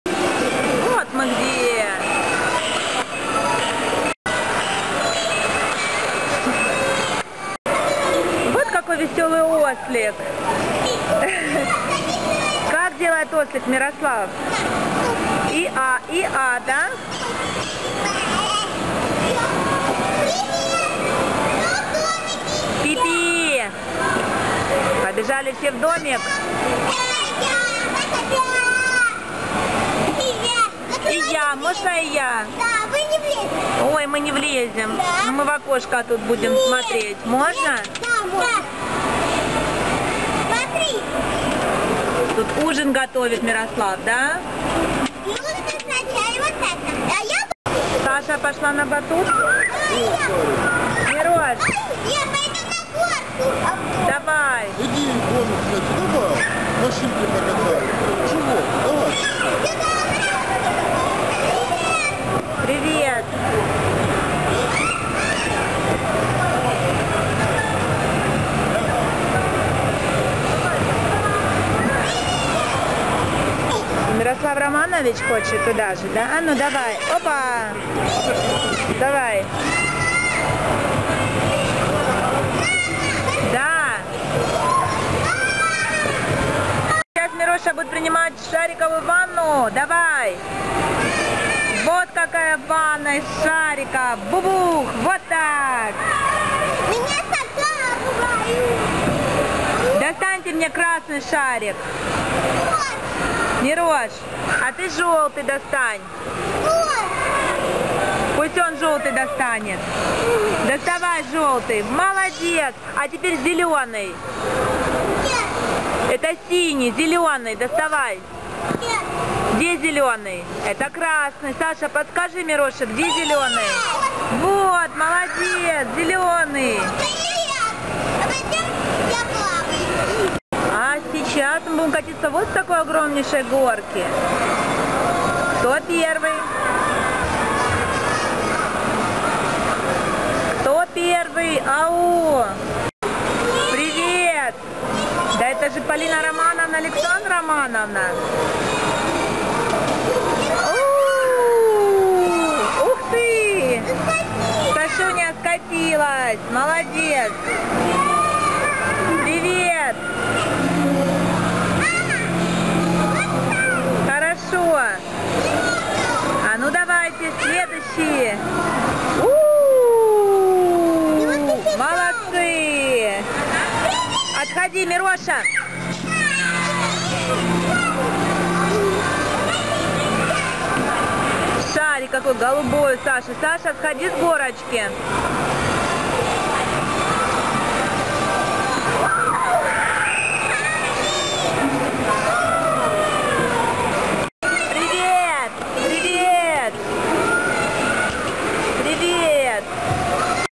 Гуляем и играем в Караване, в Игроленде. Бегаем, прыгаем на батуте, купаемся в шариках, горка.
Полина Романовна, Александра Романовна и Мирослав Романович играют в Игроленде. Прыгают на батуте, купаемся в бассейне с шариками, катаемся с горки.